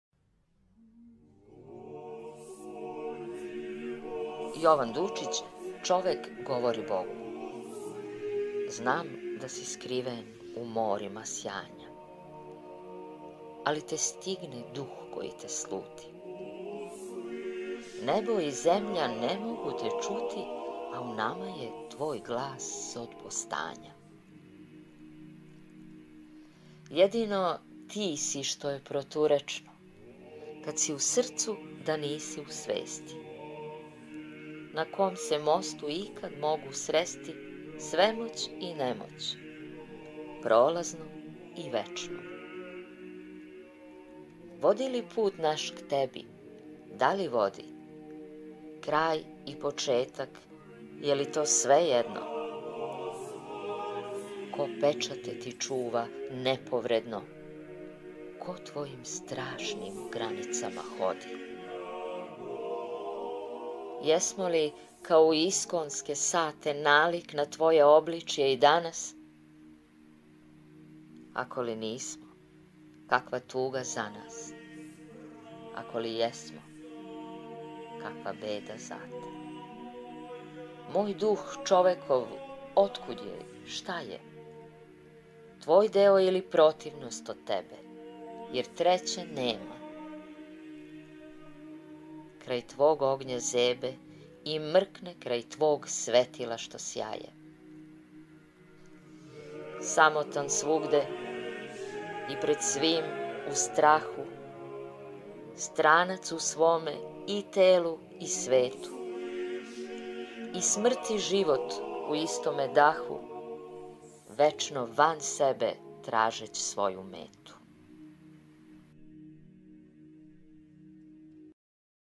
Jovan Dučić - Čovek govori Bogu
Stihove govori: Danijela Petković
Likovni prilog: Vaskrsenje Hristovo - freska u manastiru Visoki Dečani (1340)